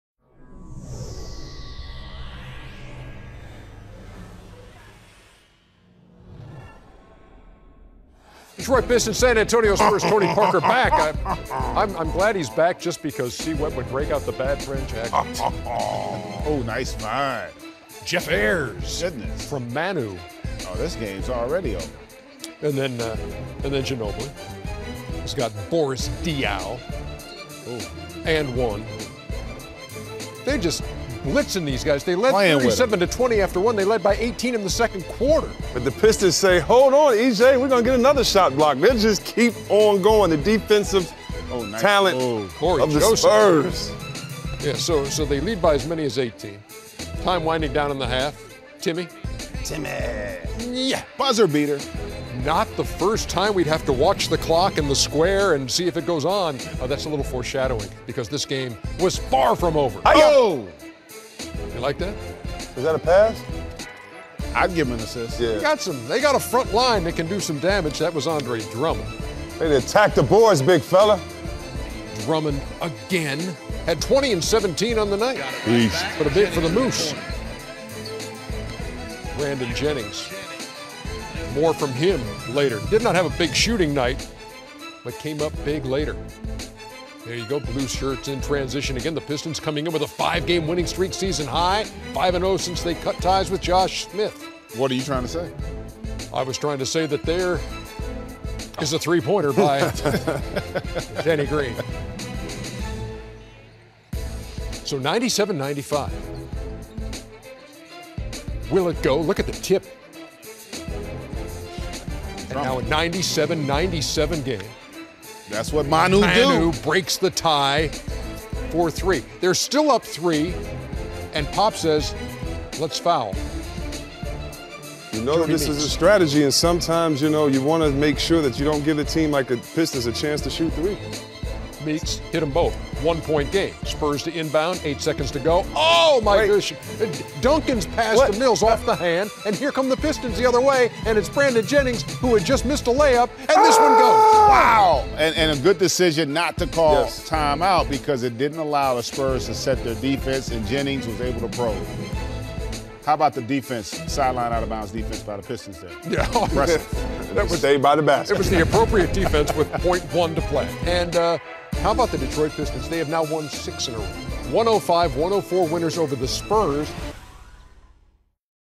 Detroit Pistons vs San Antonio Spurs | January 6, 2015 | NBA 2014-15 Season
Andre Drummond and Greg Monroe each have a double-double and the Pistons score four points in the final 11 seconds, rallying for an exciting 105-104 victory over the Spurs.